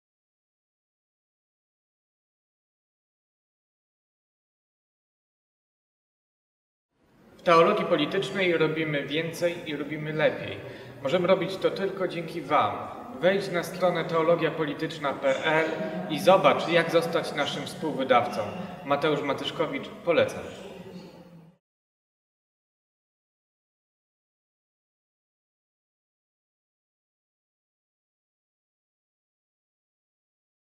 Zostań współwydawcą "Teologii Politycznej Co Miesiąc"
"Teologia Polityczna Co Miesiąc" to żywe dyskusje w nowoczesnej formie. Można nas czytać na telefonie, czytniku, tablecie, ale też na komputerze i na wydruku. Do poczytania w metrze, w pociągu, do poduchy i przy łowieniu ryb.  Zapraszamy do wsparcia!